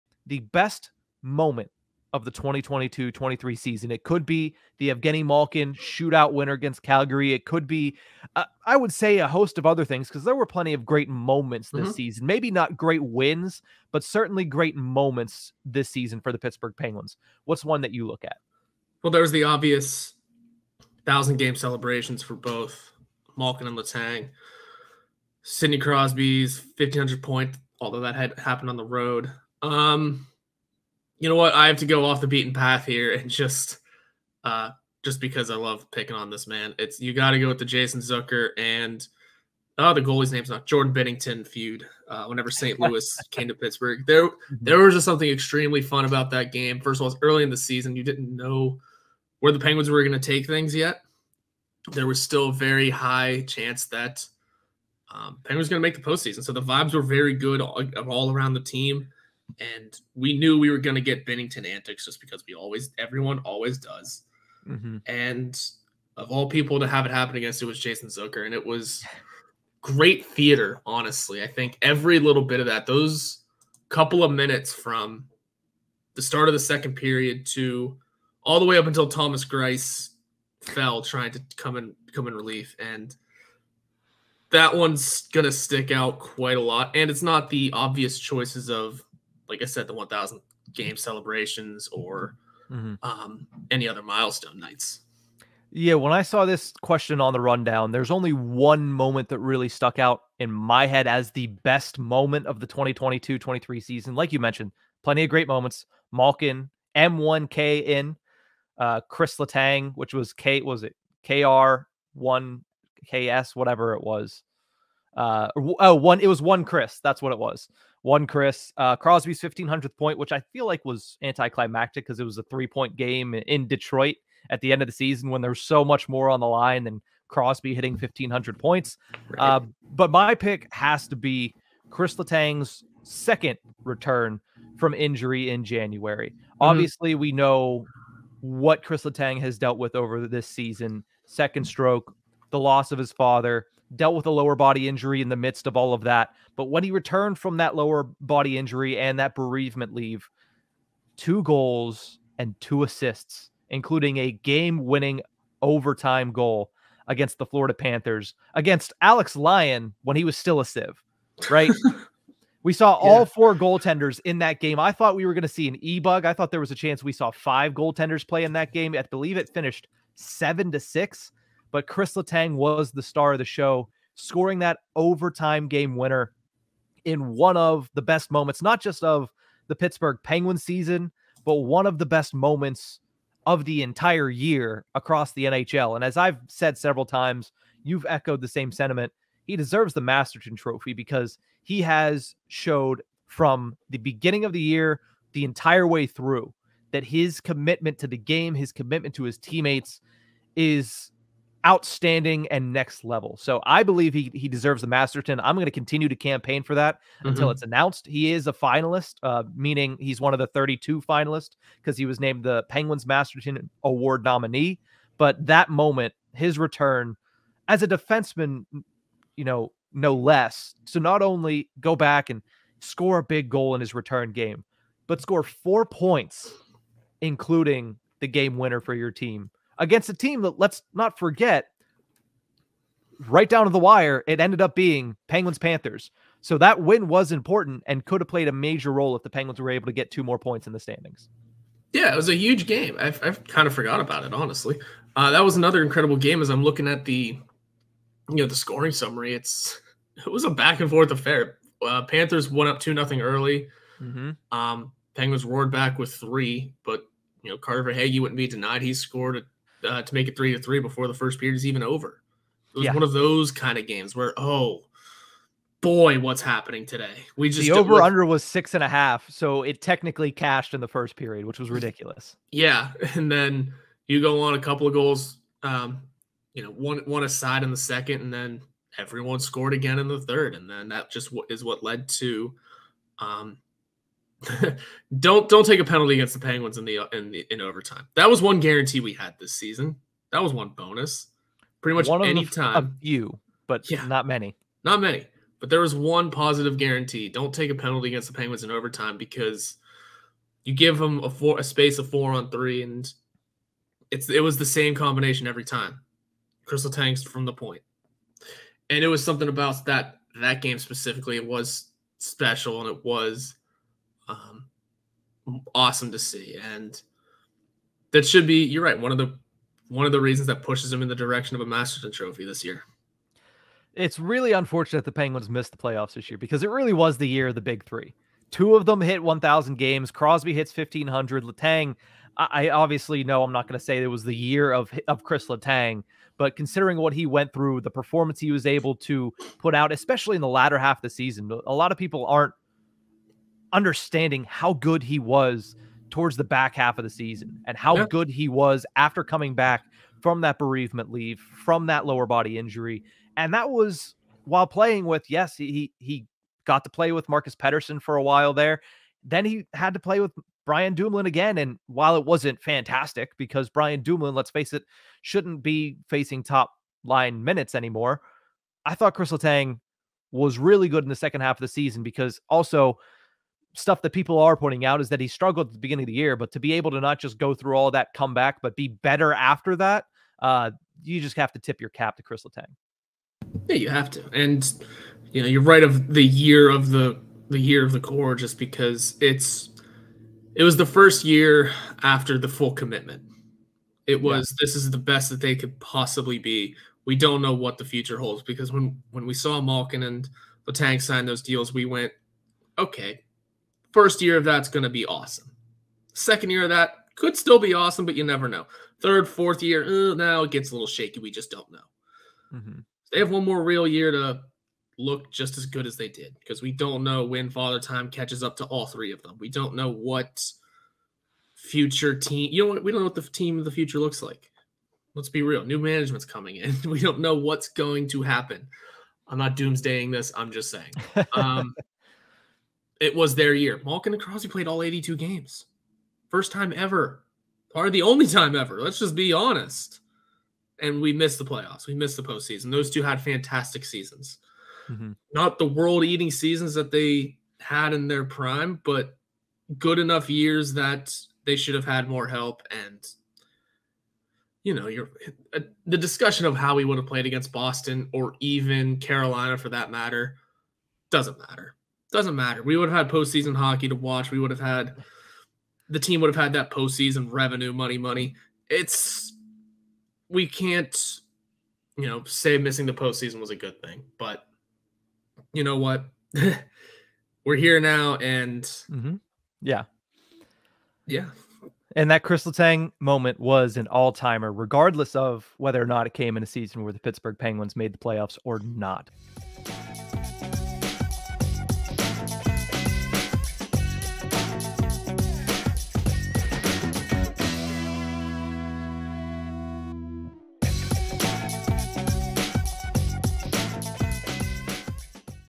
Best Moment Of The Pittsburgh Penguins Season
What was the best moment of the 2022-23 Pittsburgh Penguins season? Tune in!